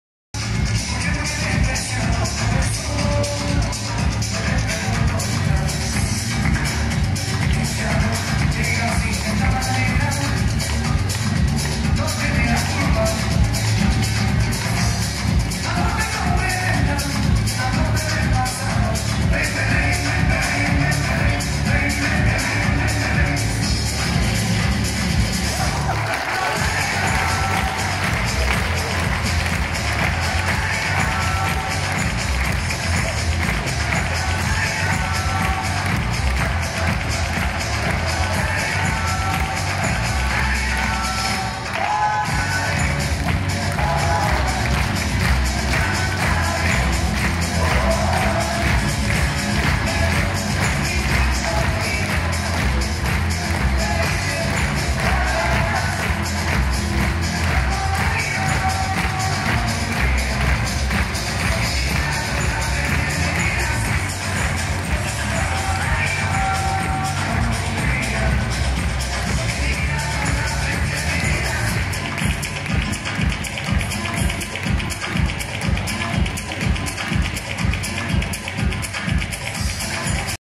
Miss Vikotoria im Weihnachtszirkus Ludwigshafen bei ihrer Tempojonglage
Miss Vikotoria zeigt Tempojonglage mit Bällen, Keulen und Ringen. Eine sehr stimmige Nummer im Weihnachtszirkus Ludwigshafen.

Weihnachtszirkus in Ludwigshafen, vor der Friedrich-Ebert Halle, ist vom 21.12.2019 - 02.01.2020